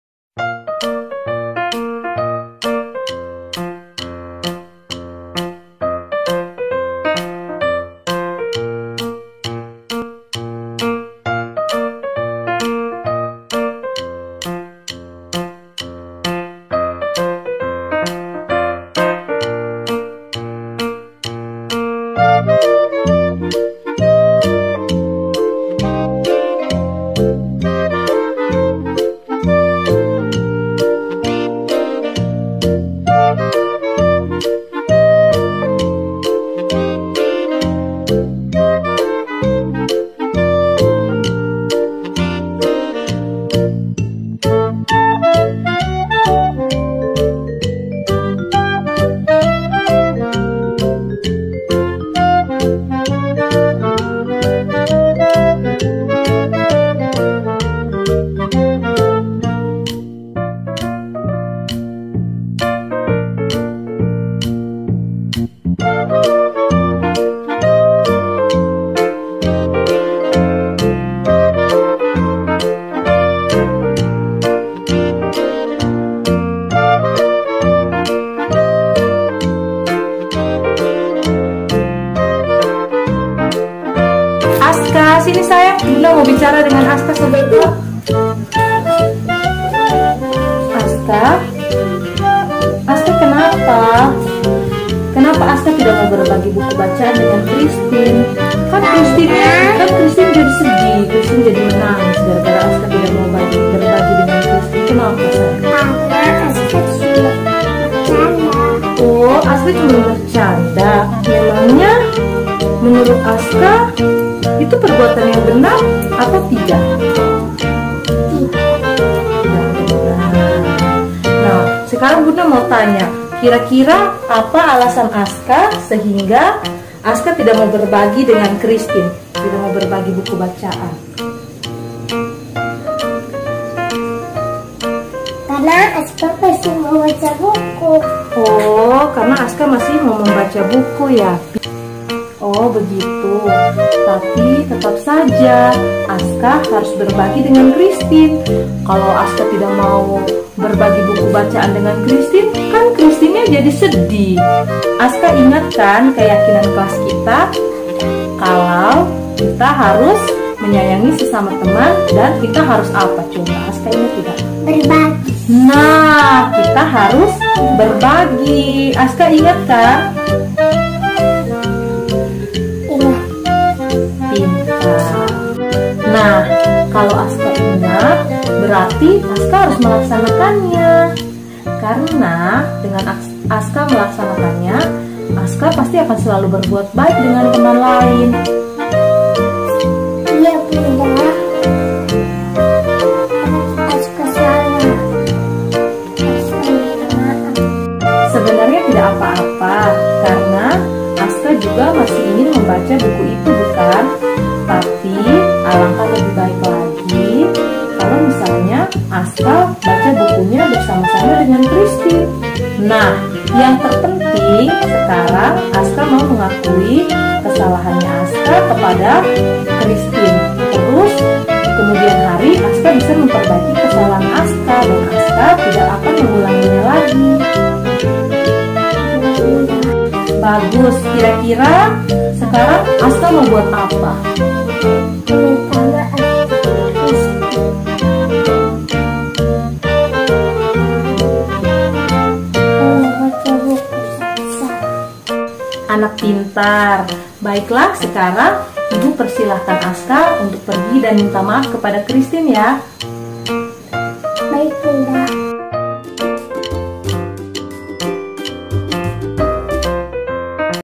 Demonstrasi Kontekstual modul 1.4.a.7.1 Segitiga Restitusi (Budaya Positif) - Anak Usia Dini
Demonstrasi Kontekstual modul 1.4.a.7.1 Segitiga Restitusi tentang budaya positif yang ada di kelas dan lingkungan sekolah

Berisi tentang studi kasus anak usia dini di Taman Kanak-kanak Sinar Pancasila Soe yang terdiri dari Skenario Kasus Azka Tunu yang tidak mau berbagi buku bacaan dengan temannya Kristin

Disini Bunda Indri memiliki posisi Kontrol sebagai menejer
Bagaimana kelanjutannya ?
Nonton sampai habis Vidio ini jangan di Skip, semoga Vidio ini bisa bermanfaat bagi kita semu

Selamat menonton dan terimakasih